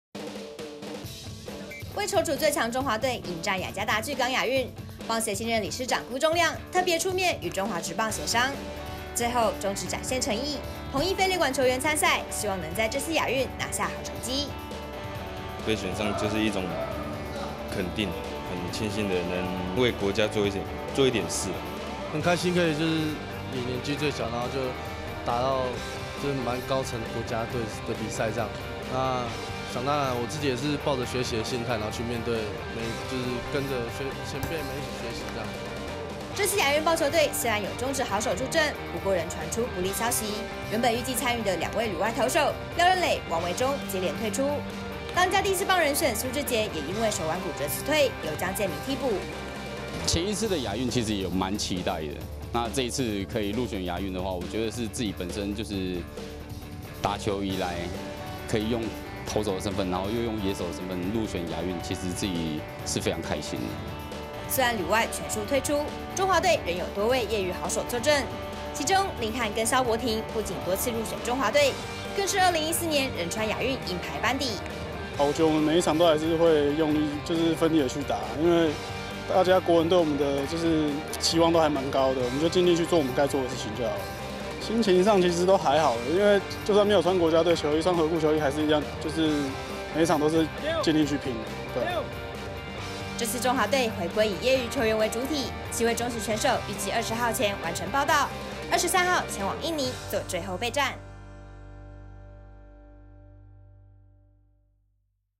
亞運》業餘配中職 中華棒球隊力拚冠軍
這次亞運棒球隊雖然有中職好手助陣，不過原本預計參與的兩位旅外投手廖任磊、王維中接連退出，當家第四棒人選蘇智傑也因手腕骨折辭退，由姜建銘替補。中華隊回歸以業餘球員為主體，23日前往印尼做最後備戰。